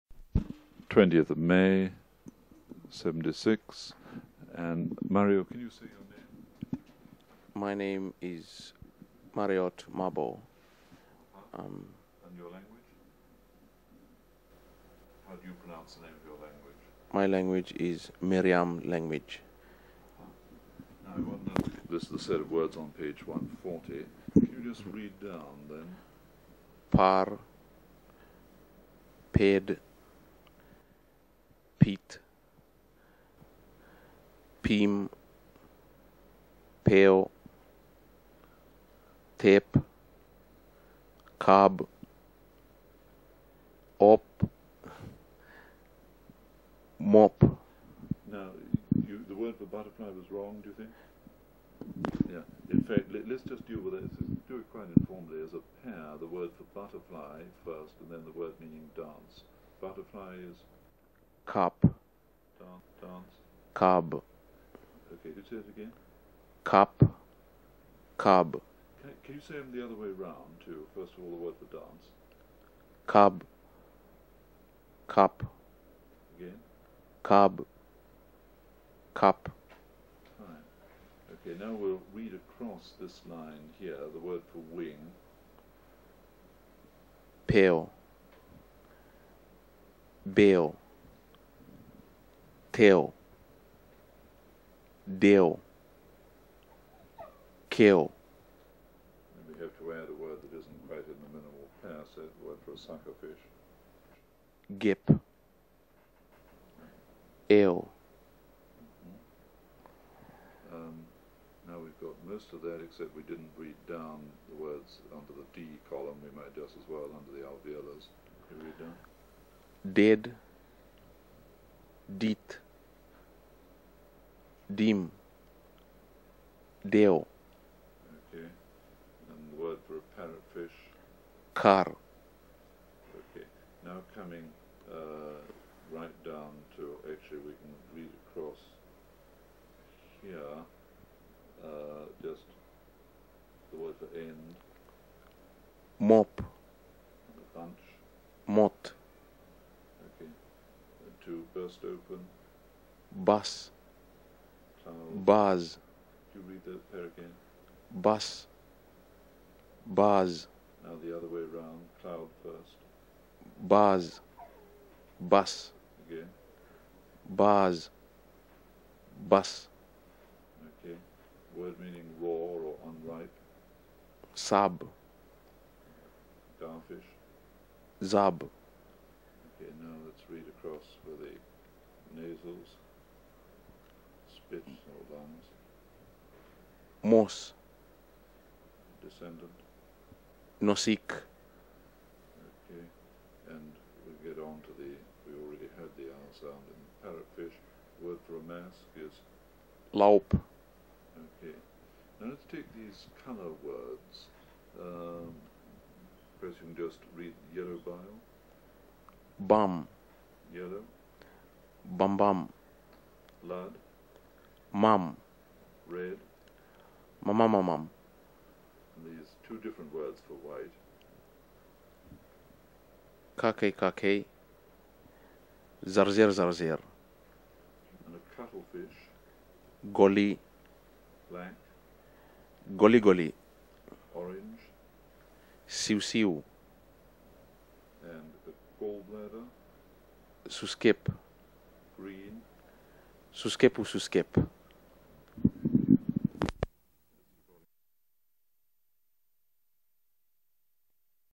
Meriam audio: ulk_word-list_1976_01
Meriam (ulk) audio: ulk_word-list_1976_01
Language family: Trans-New Guinea; Trans-Fly-Bulaka River; Trans-Fly; Eastern Trans-Fly;
Australia
Date: 5/20/1976

Description: Language Meriam.  ulk_word-list_1976_01.html, enties: 1 - 43

Contributor: Mariot Marbo
UCLA Phonetics Archive